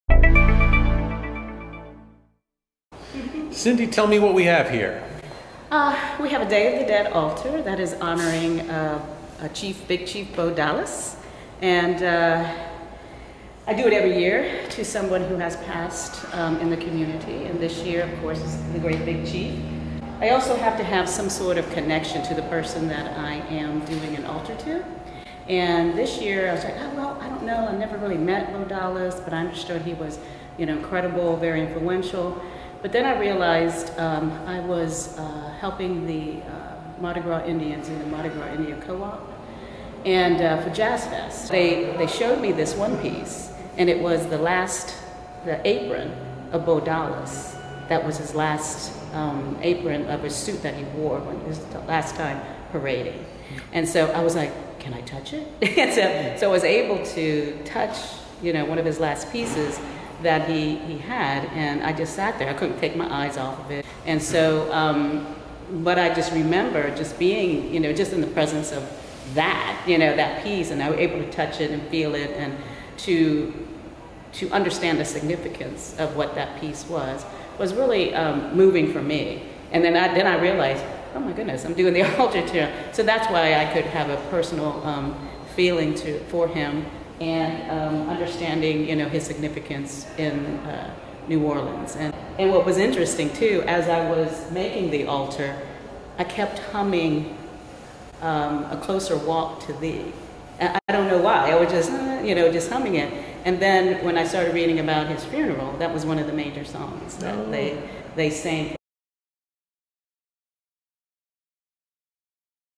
Day of the Dead altar for Big Chief Bo Dollis
Watch as artist Cynthia Damirez puts the finishing touches on her traditional Day of the Dead altar at The Ogden Museum of Southern Art. Each year, Ramirez dedicates the altar to a member of the New Orleans arts community who passed away in the p